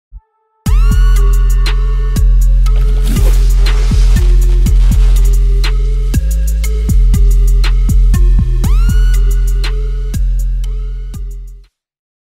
Third Coast Concentrates - Michigan Cannabis Extracts
Logo Bumper / Stinger for Third Coast Concentrates in Michigan

Seek us out if you're looking for professional marketing graphics and promotional materials.